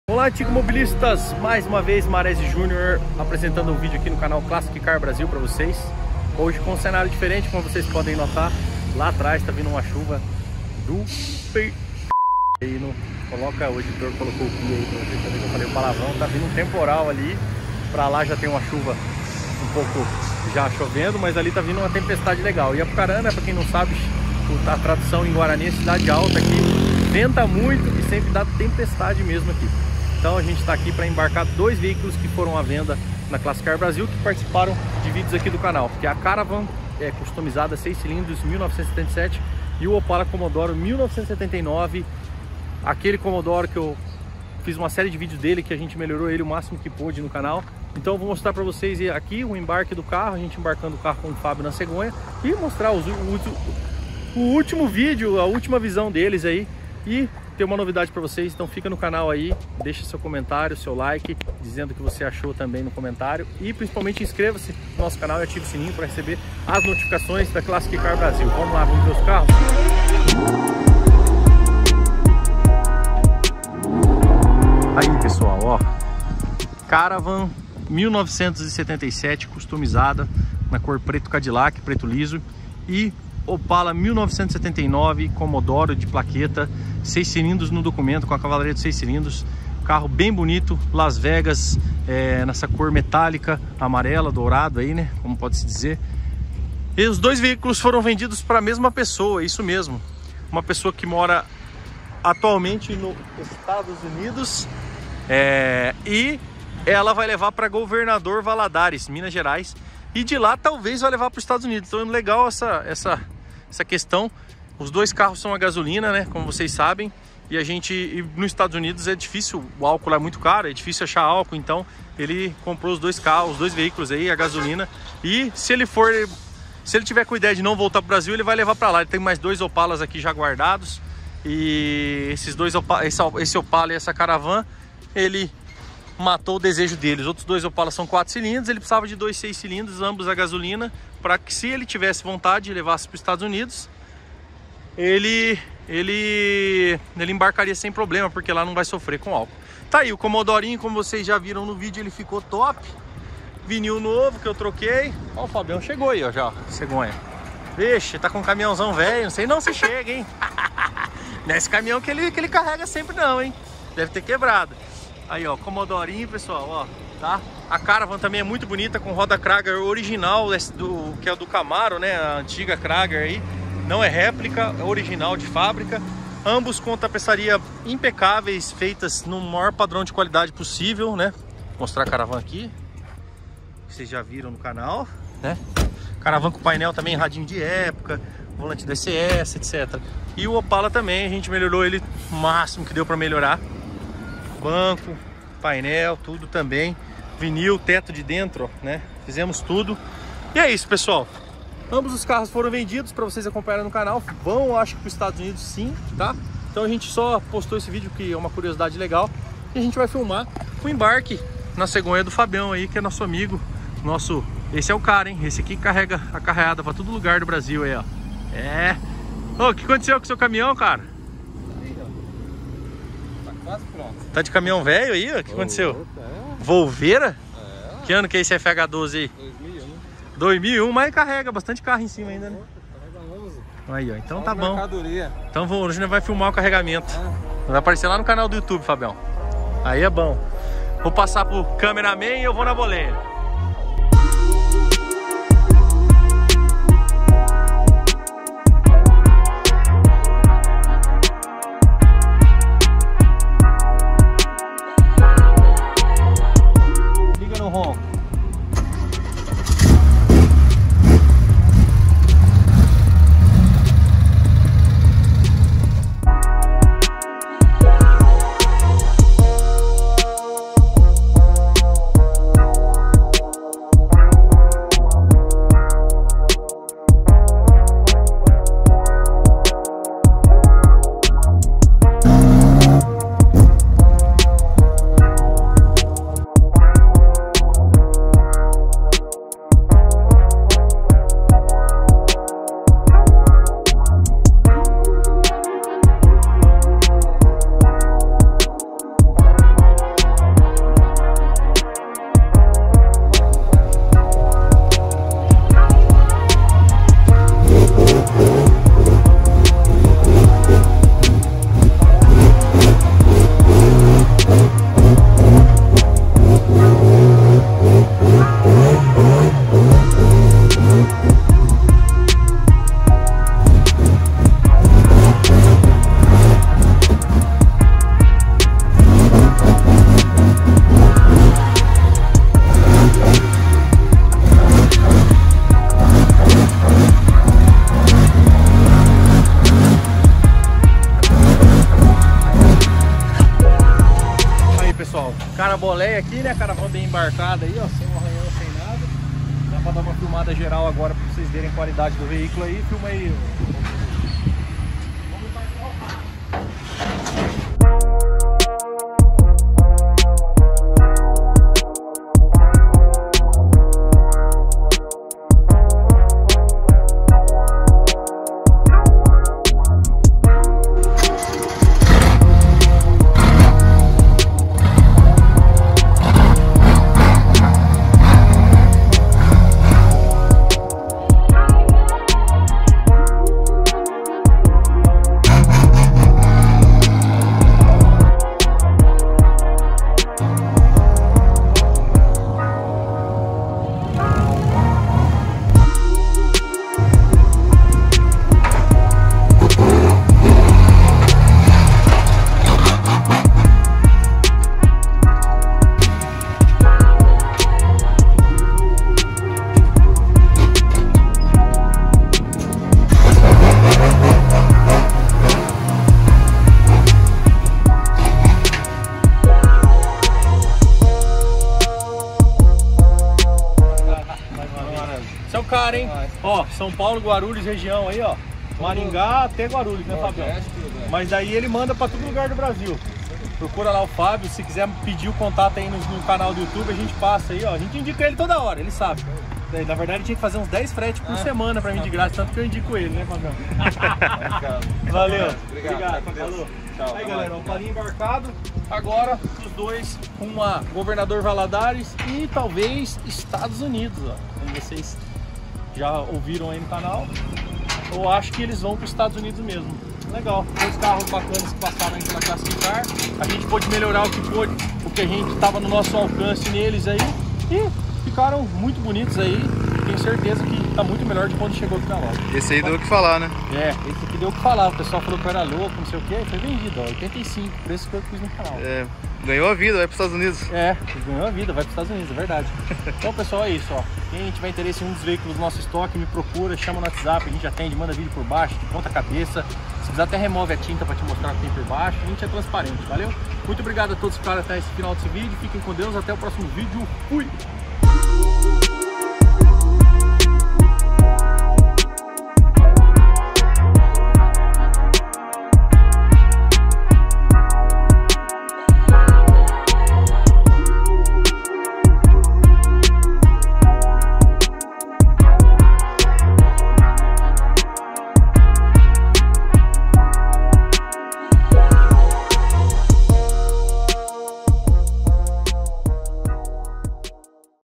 ESSES CARROS VÃO PARA OS ESTADOS UNIDOS?? / MAREZE JÚNIOR / CLASSIC CAR BRASIL
Dois clássicos que vão para o exterior e venceram na vida! Haaha!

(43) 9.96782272

"Consulte nosso estoque"